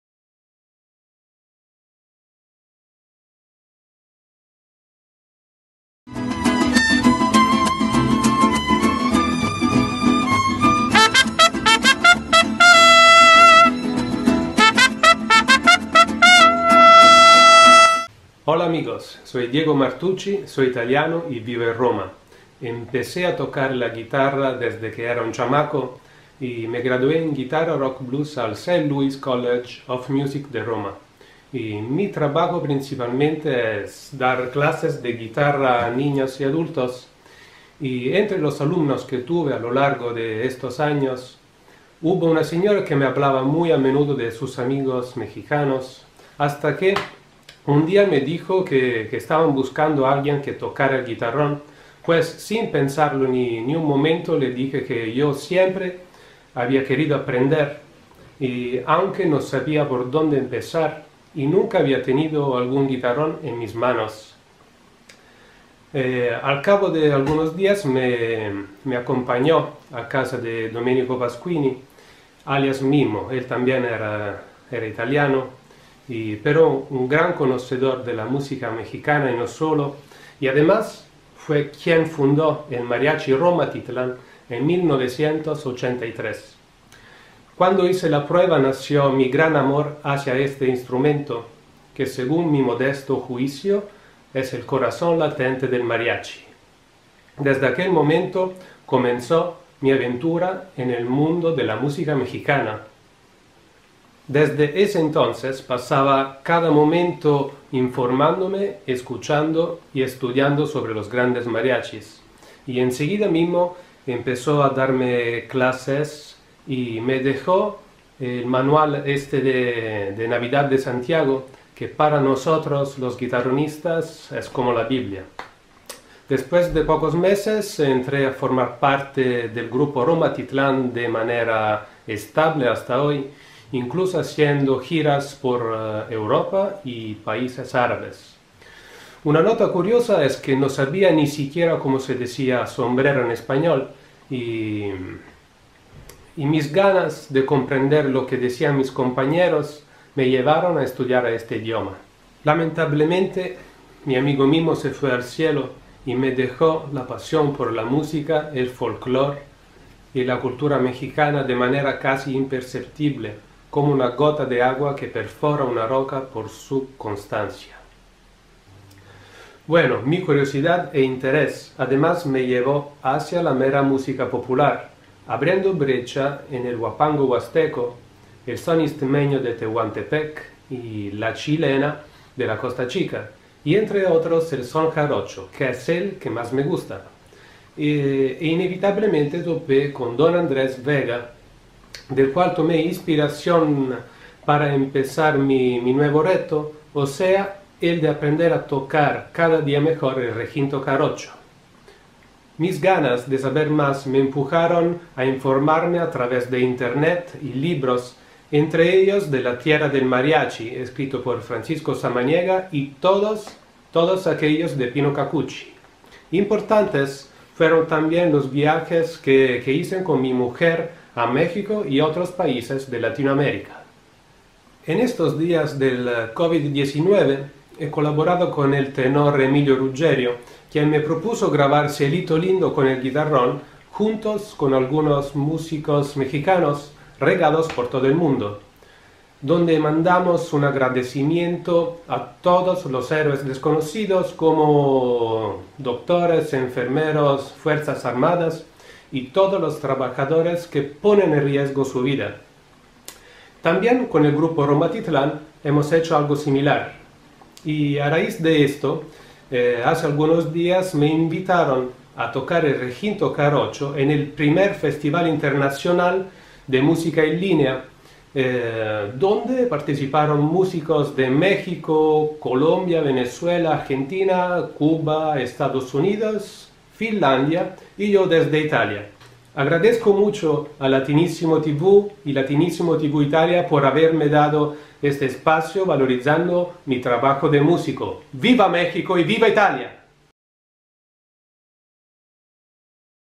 Diego Martucci, il mariachi italiano / Diego Martucci, el mariachi italiano
L'italiano Diego Martucci, tra i relatori del corso “El Mariachi en México” che si svolge online nel mese di febbraio 2021, si racconta in un breve video prodotto da Latinísimo TV Italia e sottotitolato da Puntodincontro.
El italiano Diego Martucci, uno de los ponentes del curso “El Mariachi en México” que se lleva a cabo online en febrero de 2021, habla de sí mismo en un breve video producido por Latinísimo TV Italia y subtitulado por Puntodincontro.